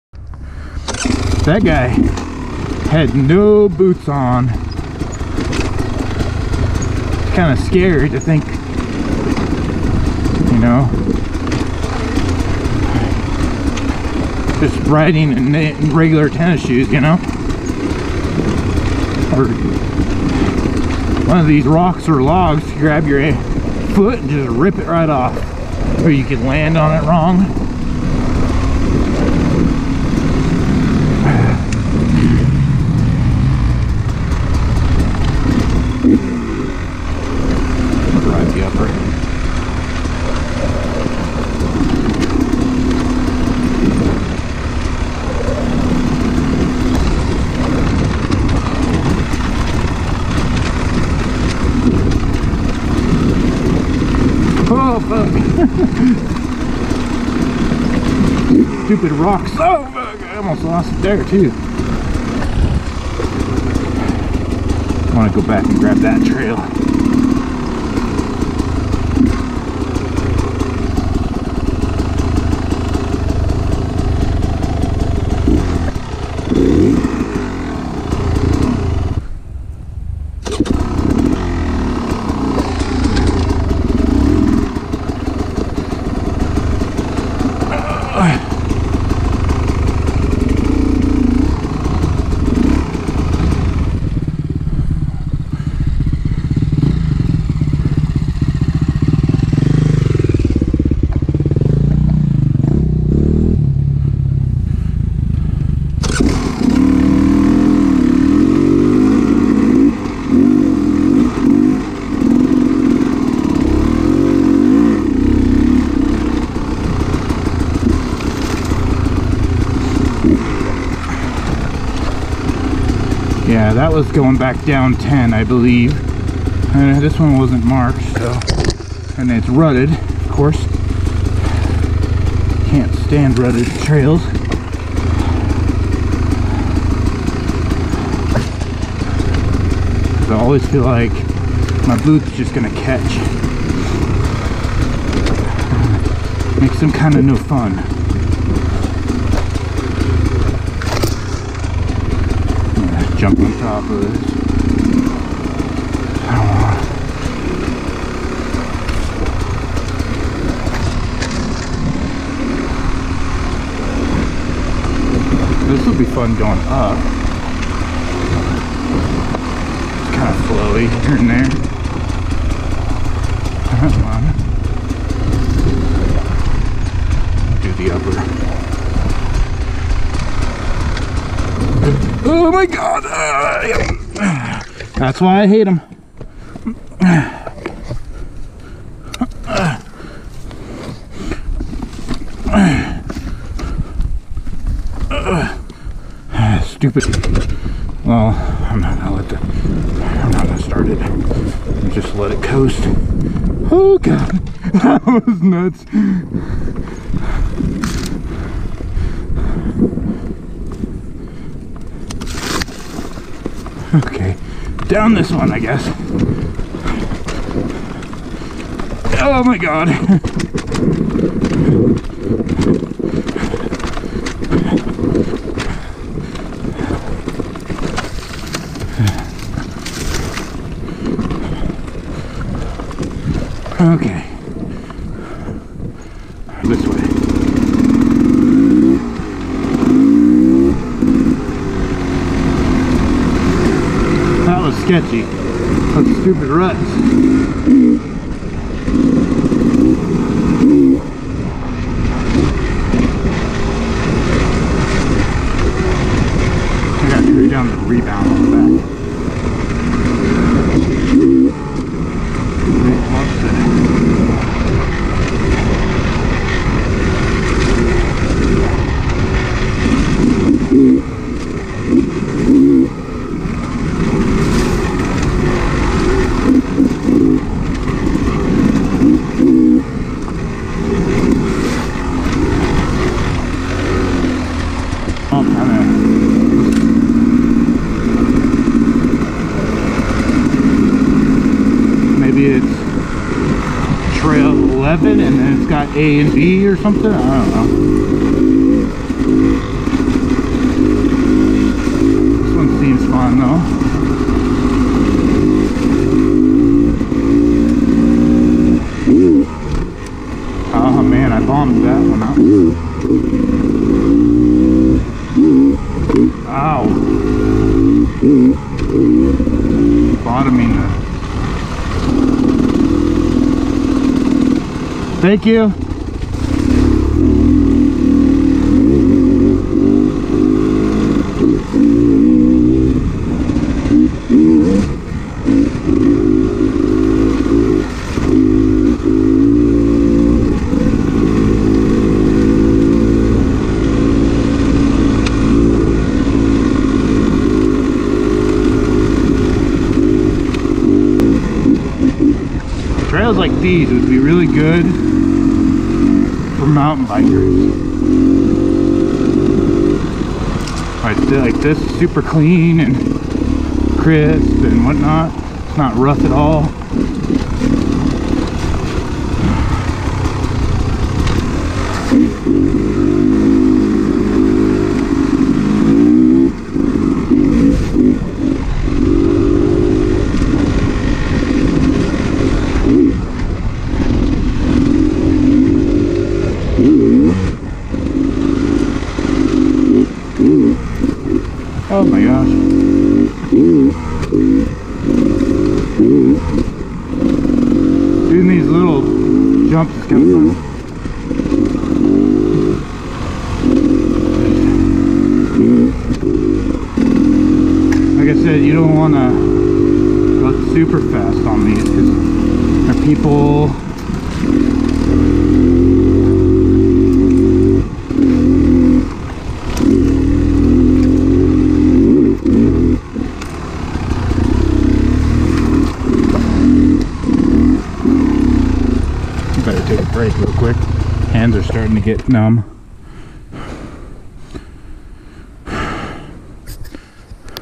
First time Ride at Canfield! Part 5 2019 Beta 300rr Race Edition
Just a ride up at Canfield Mountain in Coeur d' Alene Idaho.